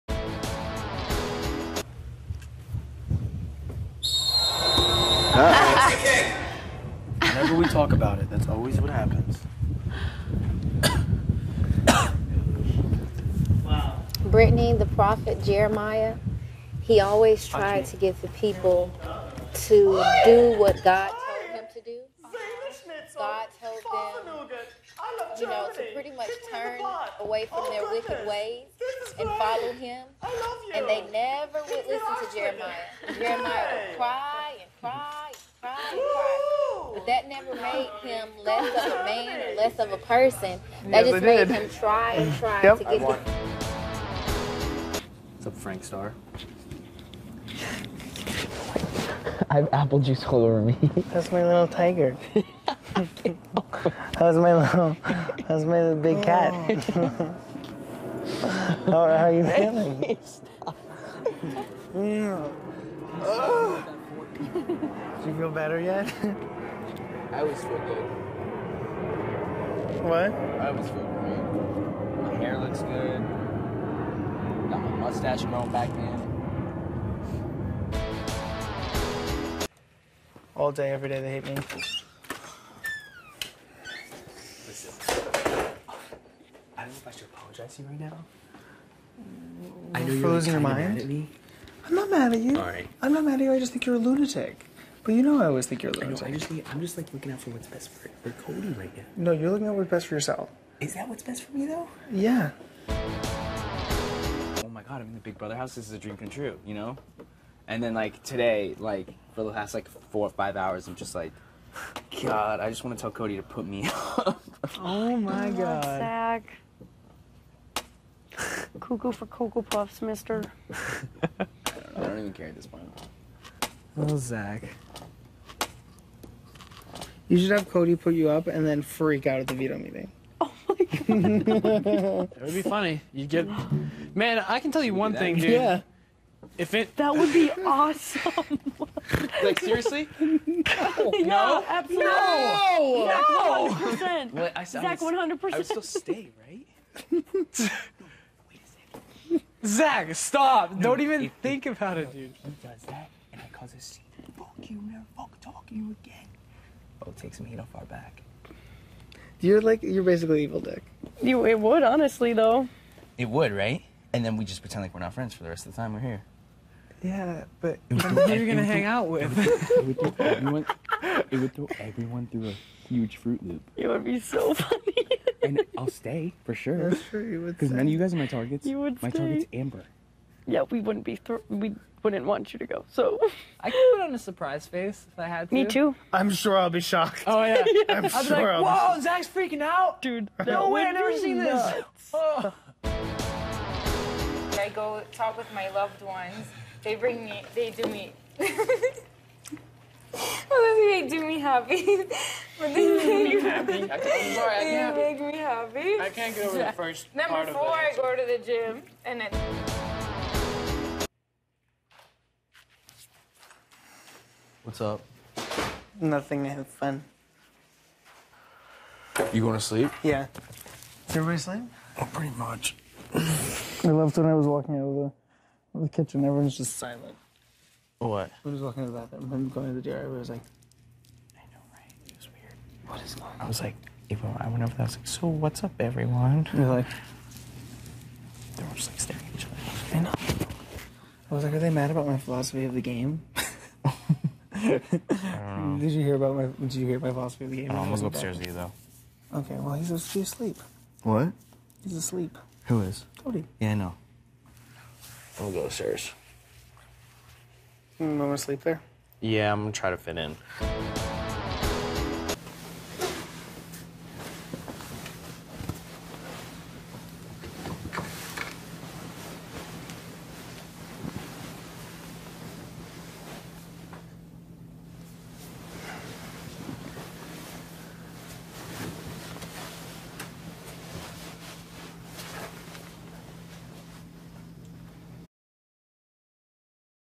REUPLOAD Zankie 7/20 - 7/21 Compilation
LOL this one. I remember this one. The beginning wasn't Zankie but I have a really weird sense of humor and i thought listening to Frankie's inappropriate yelling to Cody was so hilarious while Jocasta is giving Brittany a serious Bible-talk.

All those long Zankie vids that CBS deleted are coming back because ...because. I worked hard on that shit.